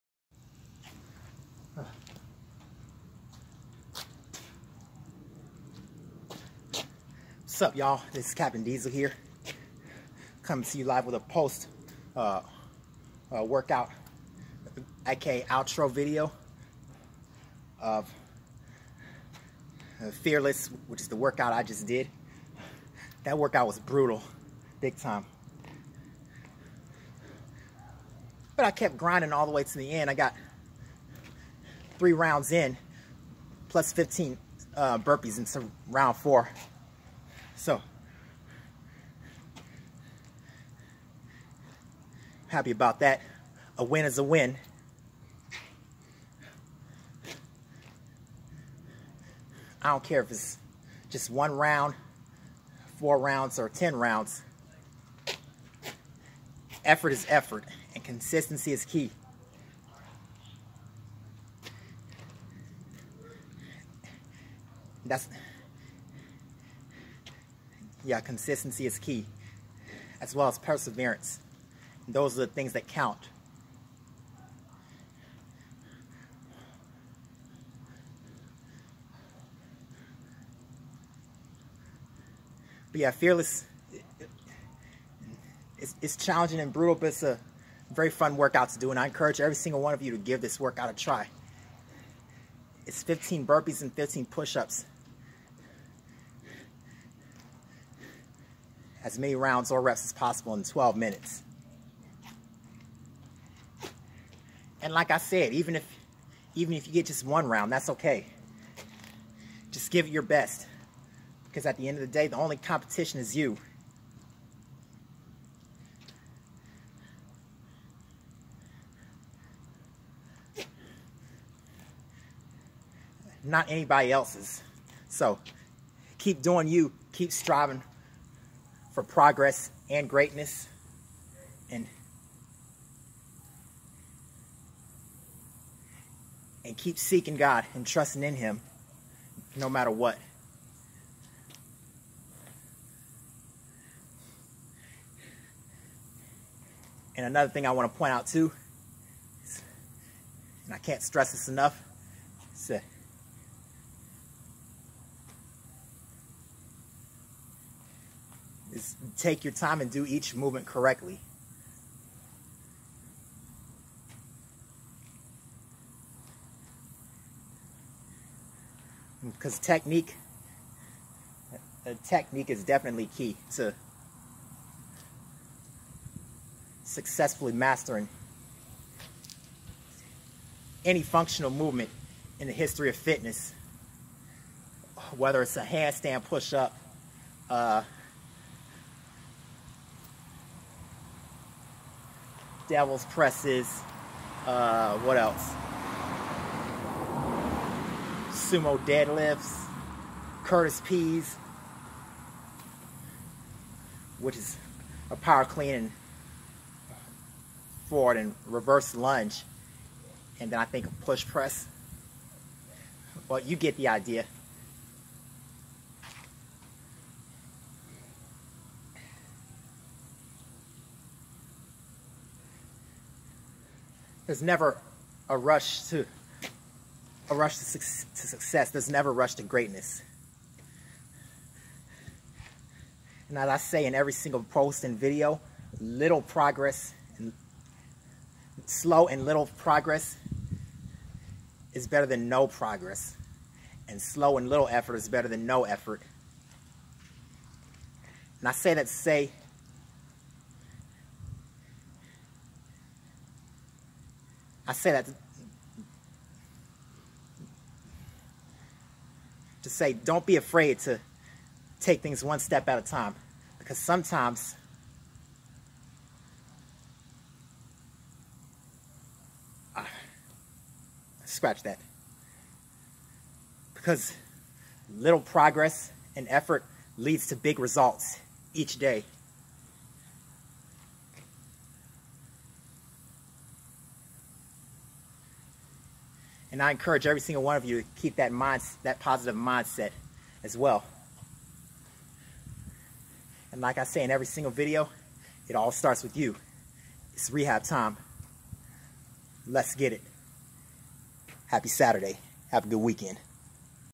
Fearless WOD Outro
I had to take a second to breathe after that tough workout, so I did a separate video of my outro. Remember, little progress adds up to big results each day, so keep striving for greatness and don't be afraid to take things one step at a time. LET'S GET IT!!!